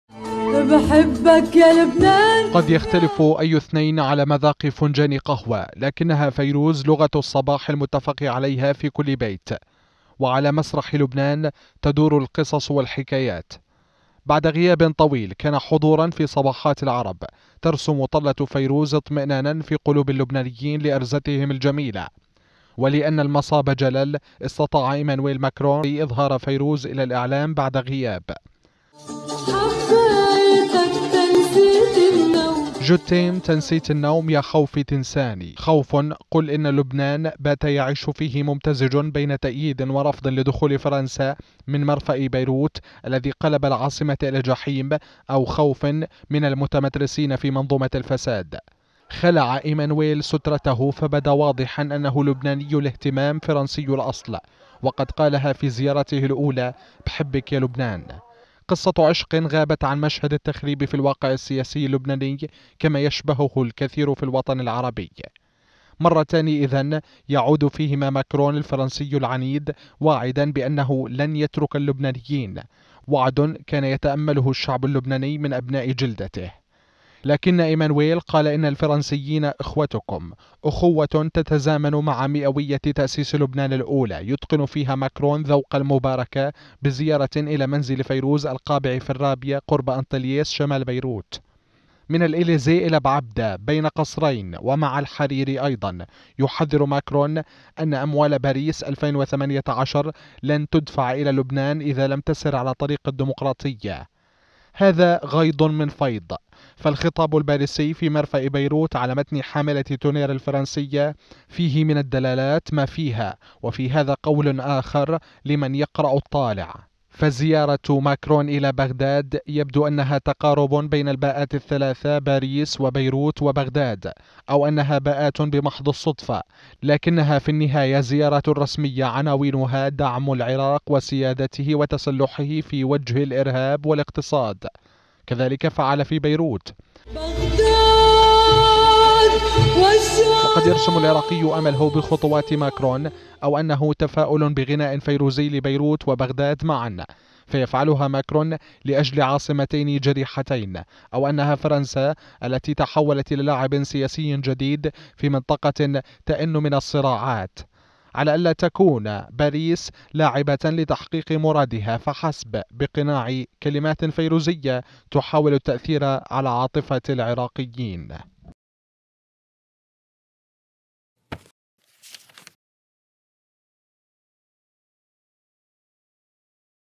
زيارة ماكرون لفيروز.. أسرار قهوة باريس على طاولتي بيروت وبغداد | تقرير
قبيل الزيارة الفرنسية لبغداد، يزور ماكرون بيروت وأرزة لبنان فيروز، زيارة قرأها كثيرون بأكثر من لغة سياسية، وبين باريس وبيروت وبغداد.. باءات ثلاثة قد تجتمع على إنجاز سياسي حقيقي أو أنها تفسير لمصالح تضعها فرنسا كلاعب رئيس في المنطقة.
فيروز ومكرون
تقرير: ناجي أبو لوز